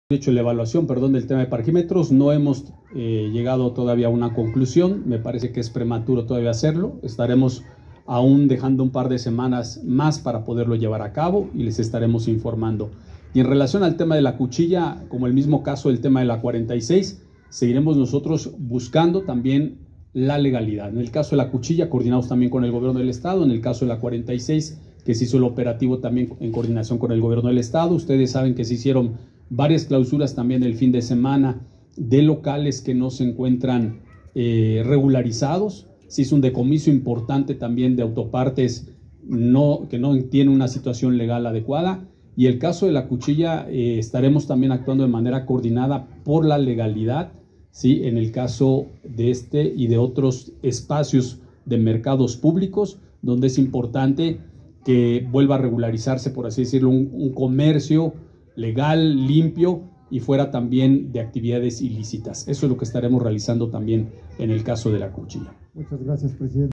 En la 46 Poniente fueron clausurados 13 establecimientos por no estar regularizados: ERP
En los operativos de la 46 Poniente fueron clausurados 13 establecimientos por no estar regularizados y se decomisaron autopartes, revela el edil capitalino, Eduardo Rivera